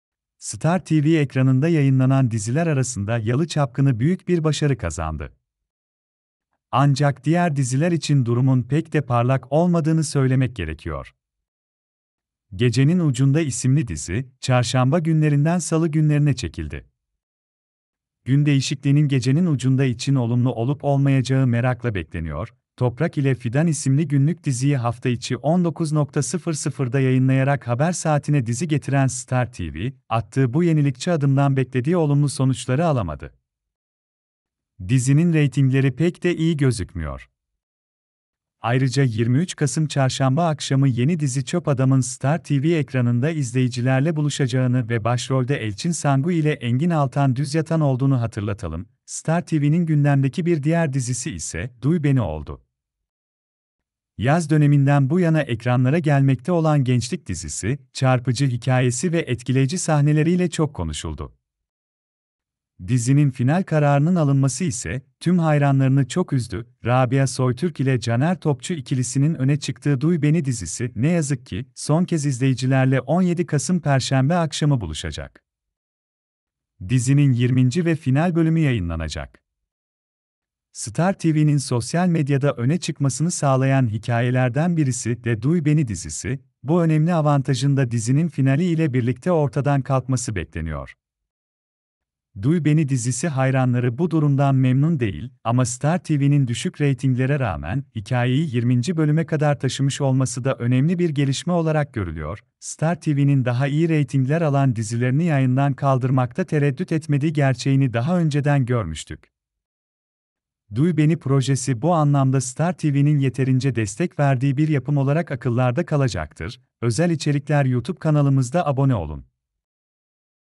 Duy Beni Dizisinden Flaş Final Kararı
Star TV ekranında yayınlanan diziler arasında Yalı Çapkını büyük bir başarı kazandı. Ancak diğer diziler için durumun pek de parlak olmadığını söylemek gerekiyor. Gecenin Ucunda isimli dizi, çarşamba günlerinden salı günlerine çekildi. Gün değişikliğinin Gecenin Ucunda için olumlu olup olmayacağı merakla bekleniyor.Toprak ile Fidan isimli günlük diziyi hafta içi 19.00’da yayınlayarak haber saatine dizi getiren Star TV, attığı bu yenilikçi adımdan beklediği olumlu sonuçları alamadı. Dizinin reytingleri pek de iyi gözükmüyor. Ayrıca 23 Kasım Çarşamba akşamı yeni dizi Çöp Adam’ın Star TV ekranında izleyicilerle buluşacağını ve başrolde Elçin Sangu ile Engin Altan Düzyatan olduğunu hatırlatalım.Star TV’nin gündemdeki bir diğer dizisi ise Duy Beni oldu… Yaz döneminden bu yana ekranlara gelmekte olan gençlik dizisi, çarpıcı hikayesi ve etkileyici sahneleriyle çok konuşuldu. Dizinin final kararının alınması ise tüm hayranlarını çok üzdü.Rabia Soytürk ile Caner Topçu ikilisinin öne çıktığı Duy Beni dizisi ne yazık ki, son kez izleyicilerle 17 Kasım Perşembe akşamı buluşacak. Dizinin 20. ve final bölümü yayınlanacak. Star TV’nin sosyal medyada öne çıkmasını sağlayan hikayelerden birisi de Duy Beni dizisi.Bu önemli avantajın da dizinin finali ile birlikte ortadan kalkması bekleniyor. Duy Beni dizisi hayranları bu durumdan memnun değil ama Star TV’nin düşük reytinglere rağmen hikayeyi 20. bölüme kadar taşımış olması da önemli bir gelişme olarak görülüyor.Star TV’nin daha iyi reytingler alan dizilerini yayından kaldırmakta tereddüt etmediği gerçeğini daha önceden görmüştük.